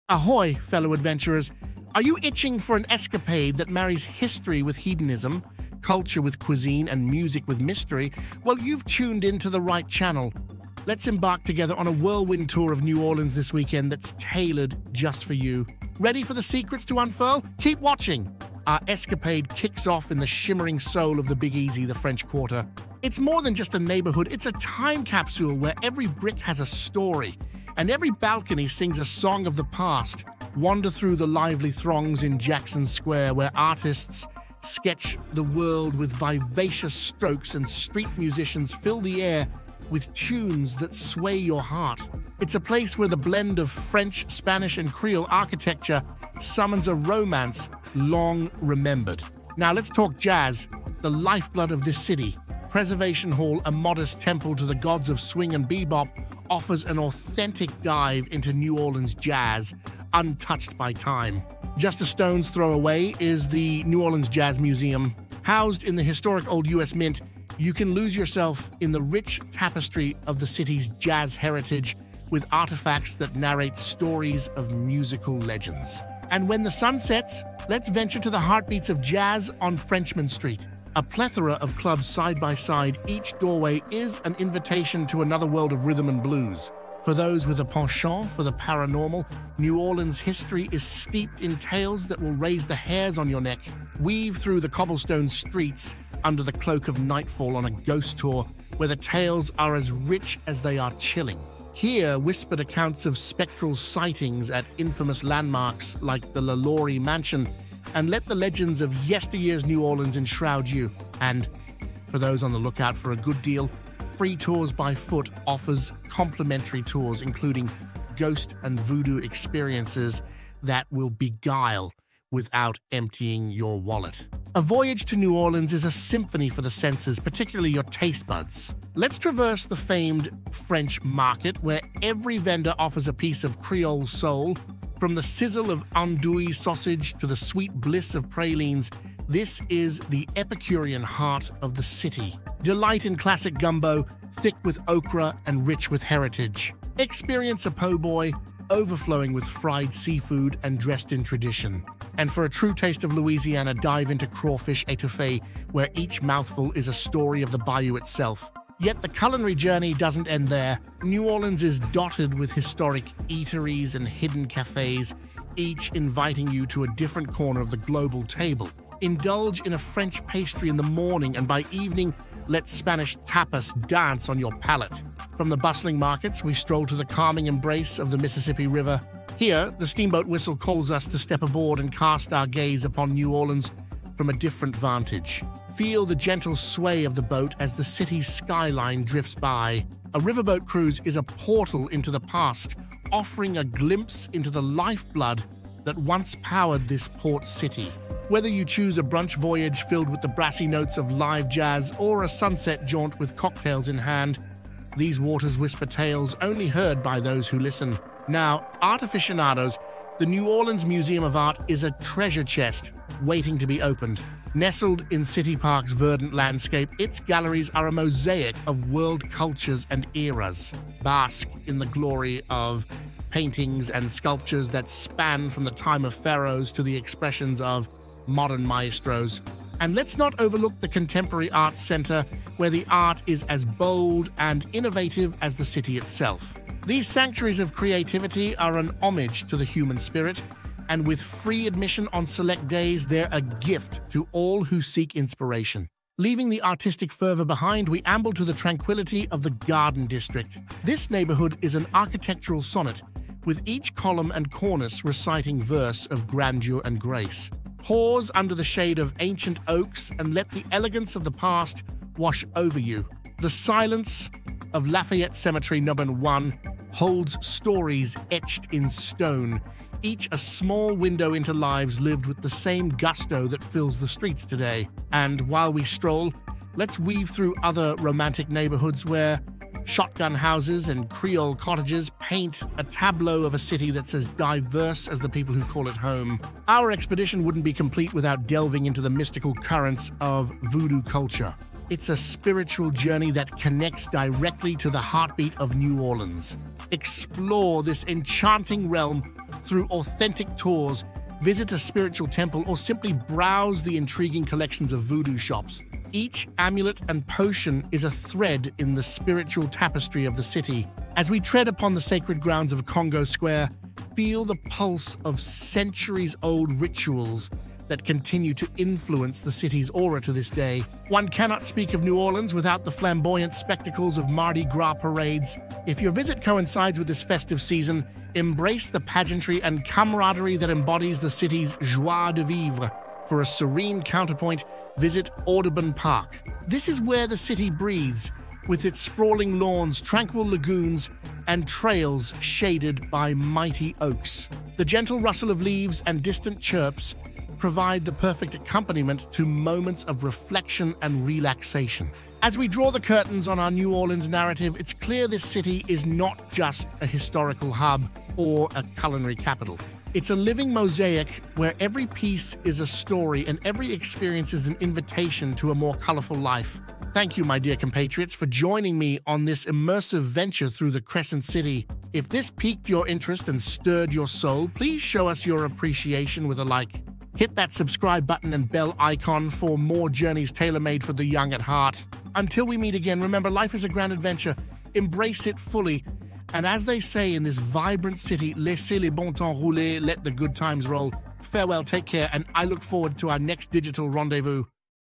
Fun & Fabulous: Things to Do in New Orleans This Weekend
Join us on an enchanting journey through New Orleans, designed for the young at heart! 🎺 In our latest video, we explore the vibrant French Quarter, where history and culture dance on every corner. Feel the rhythm of the city with live jazz at the iconic Preservation Hall 🎷 and get swept away by haunting tales on a ghost tour 👻. Indulge your taste buds with culinary treasures at the French Market 🍲, and take a serene cruise on the Mississippi River 🚢. Uncover the mesmerizing world of contemporary art at NOMA 🎨, and meander through the lush Garden District 🌳. Dive deep into the mystical voodoo culture 🔮 and, if you're lucky, join the jubilation of a Mardi Gras parade 🎭. Whether you're seeking romance, adventure, or simply a relaxing weekend, New Orleans has it all. 🎷🎉